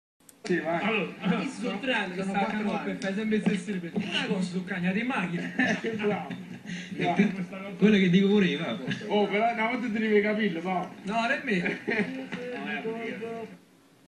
Napoli, super siparietto tra Cannavaro e un cameraman: "Semp o stess!"
CASTEL VOLTURNO (CE) - A Napoli si vive un momento particolarmente sereno. Lo scorso 3 marzo, mentre Mauro Ielardi, cameraman di Sky, filmava la consueta uscita dagli spogliatoi dei calciatori azzurri in procinto di iniziare l'allenamento del pomeriggio, ecco come e' simpaticamente intervenuto il capitano Paolo Cannavaro con un perfetto napoletano: "Chist' so 3 anni che sta ca' ngopp e fa semp' e stessi riprese! So cagnat' sol e machin!". Pronta la risposta in un clima del tutto goliardico: "E pure hai ragione". Infine la straordinaria chiosa dell'ottimo inviato Francesco Modugno: "Paolo ti sbagli, qualche anno fa avevi i capelli!". E il difensore: "Nemmeno!". Ecco il video che "Napoli Magazine" vi ripropone: